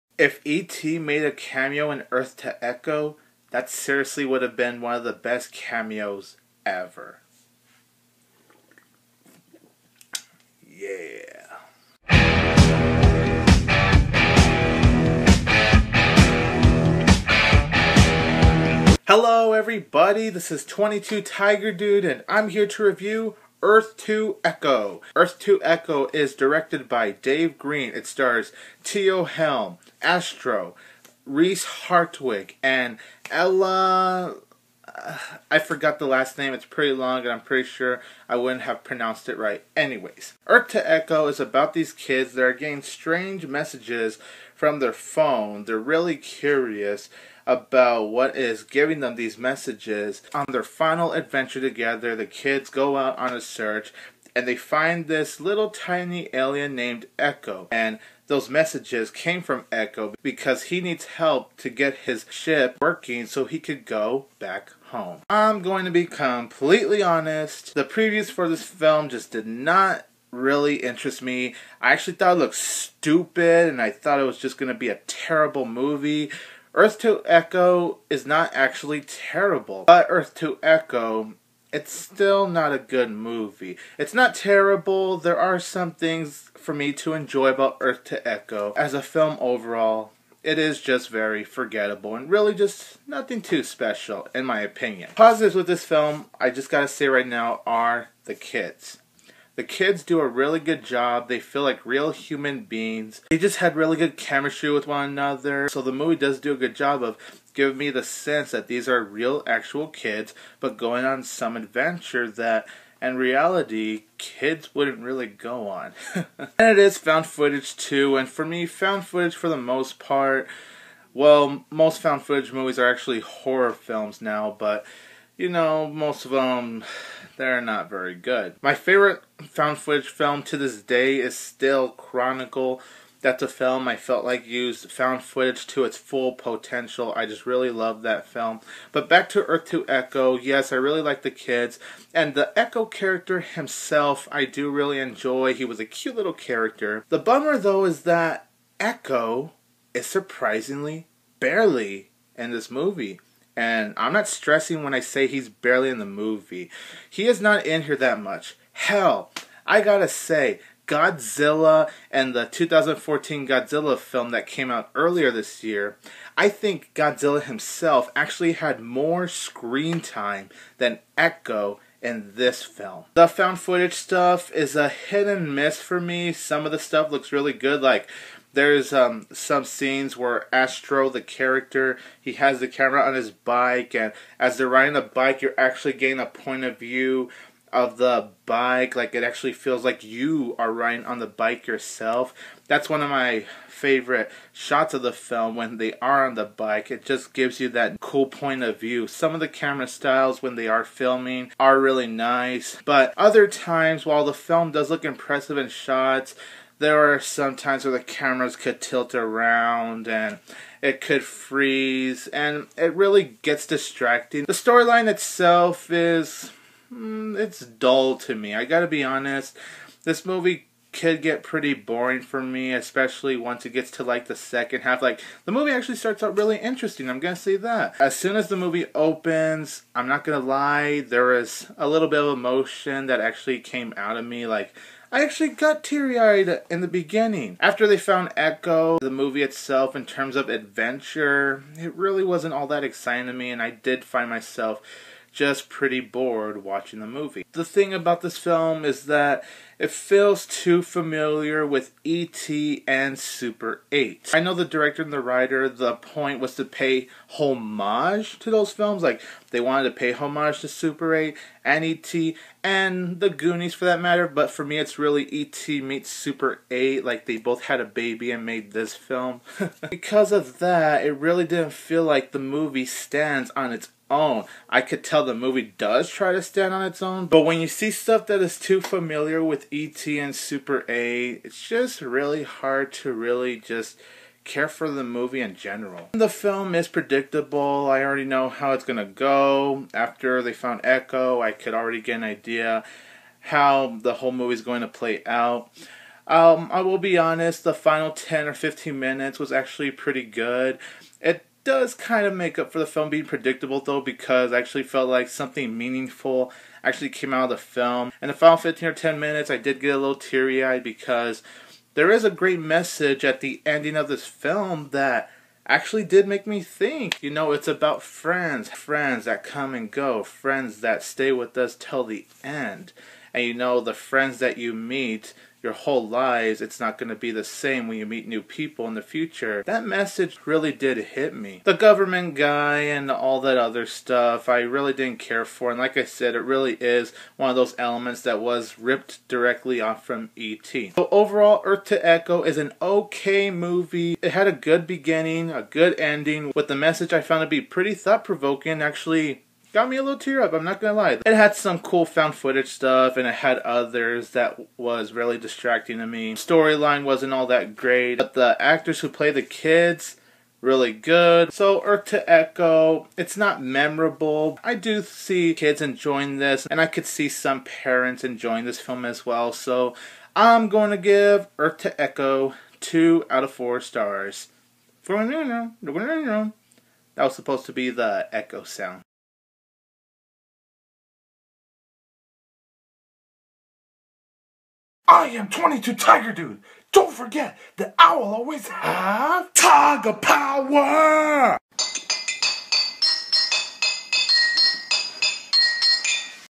Earth To Echo movie review
22tigerdude reviews the sci-fi family adventure film directed by Dave Green. Starring Teo Halm, Astro, Reese Hartwig, and Ella Wahlestedt. What are his thoughts on this film? find out here.